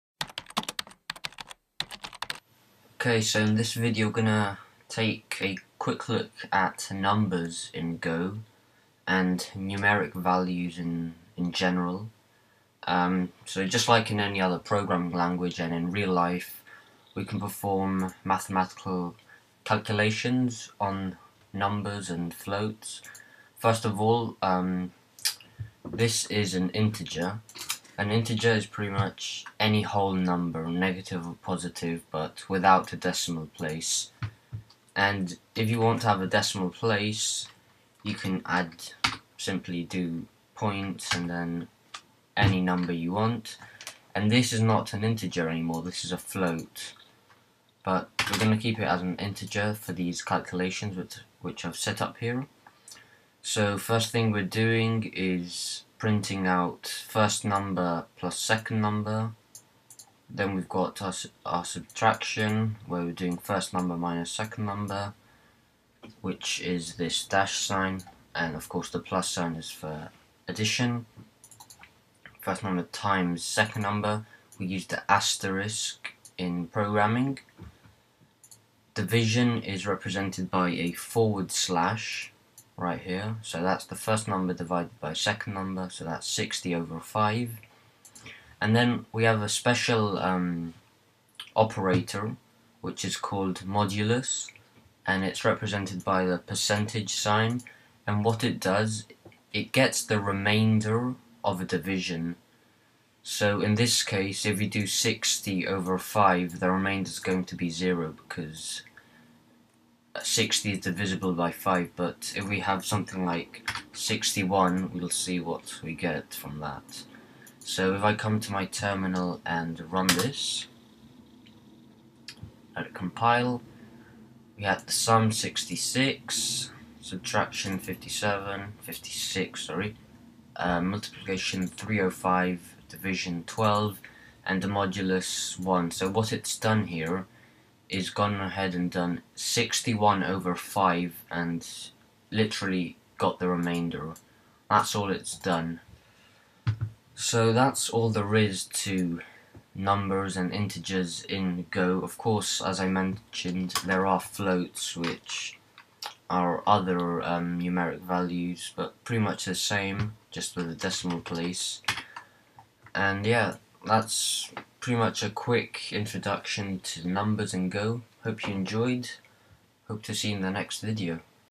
Go Programming (golang) - 03: Numbers and Math
Learn Go programming, from beginner to pro!

We learn how to use numbers and integers to perform mathematical operations in Go.